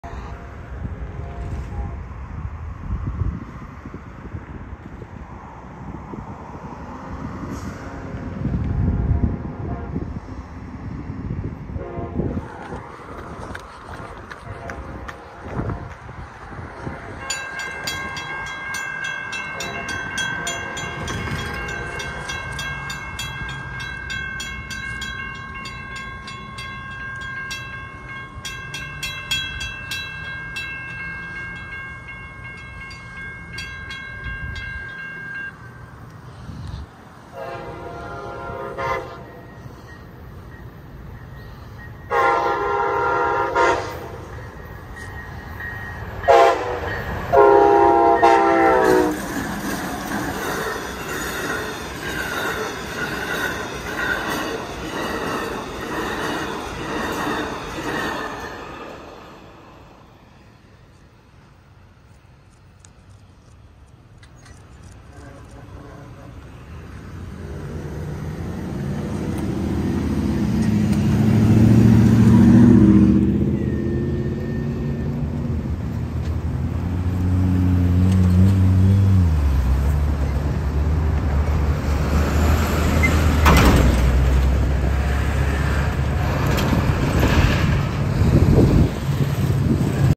Amtrak 91 goes by Seffner, FL
Amtrak 91 - New York City to Miami

Next station: Tampa, FL